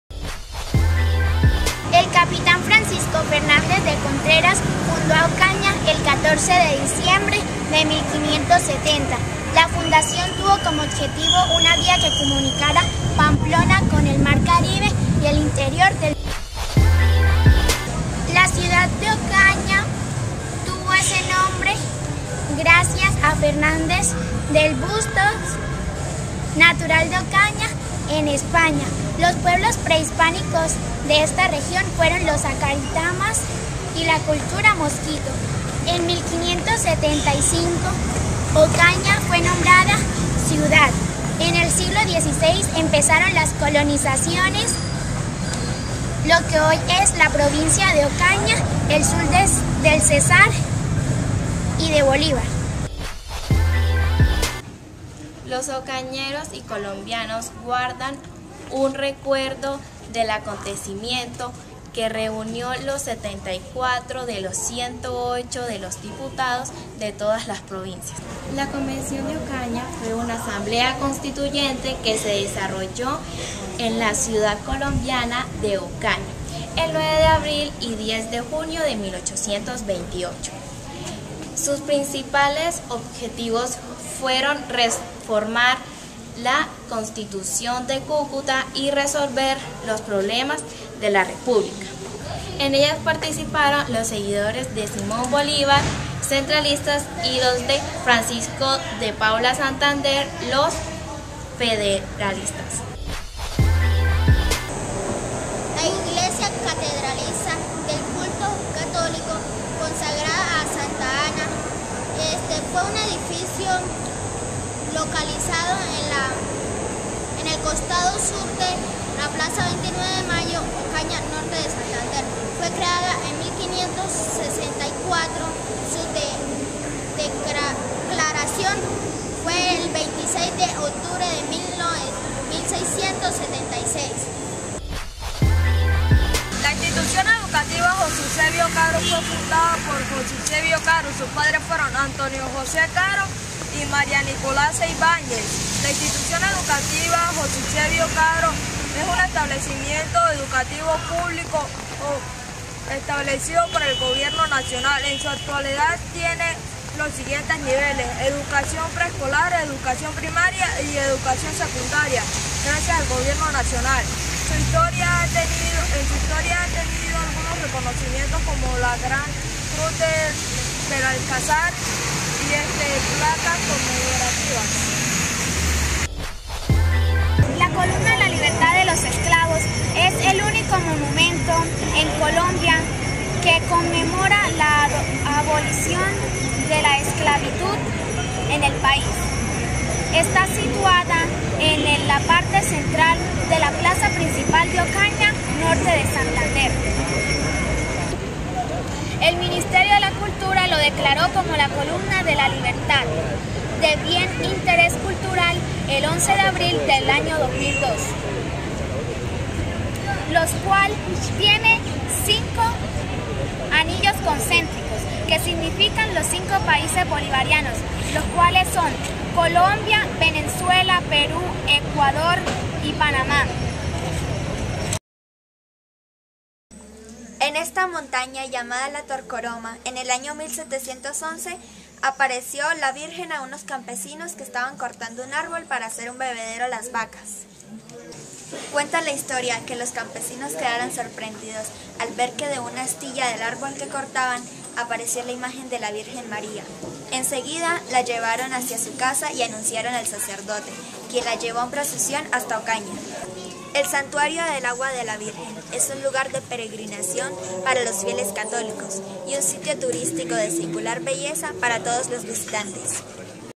Historia de Ocaña, Norte de Santander. 7-1
Trabajo realizado por estudiantes del Colegio Caro en sus primeras prácticas alrededor de la comunicación social.